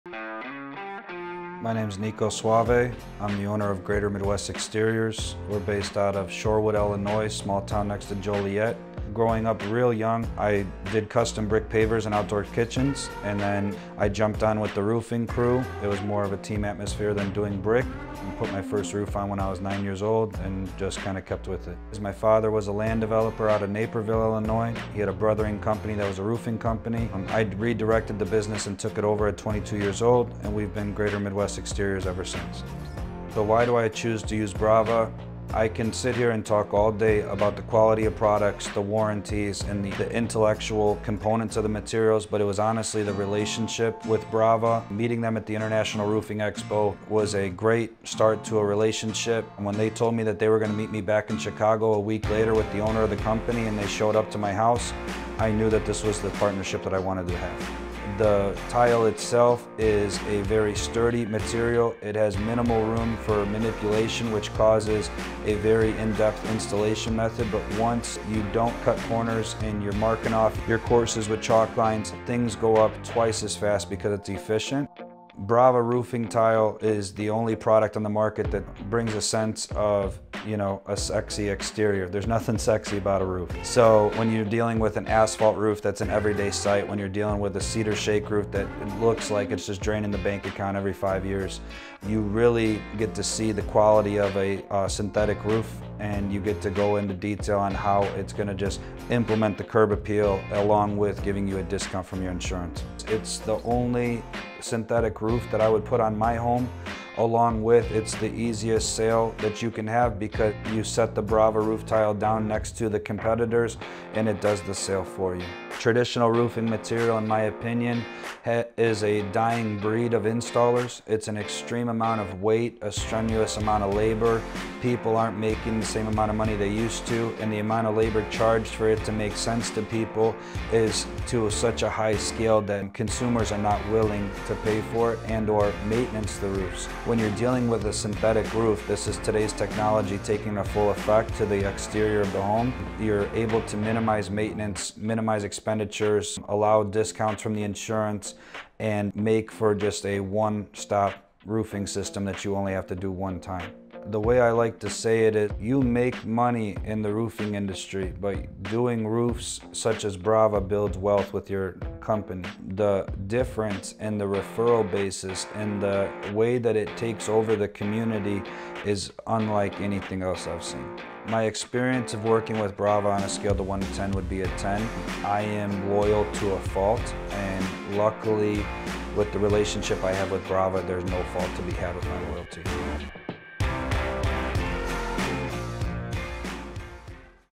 Why Contractors Choose Brava: Unveiling the Benefits with Nico Soave
Get the Inside Scoop!  We recently sat down with Nico Soave, owner of Greater Midwest Exteriors in Shorewood, IL, to hear why he loves using Brava Roof Tile for his clients.  In this interview, Nico dives deep into the benefits of Brava and shares his experience with various Brava installations.  Discover what makes Brava the perfect choice for homeowners in the Chicagoland area!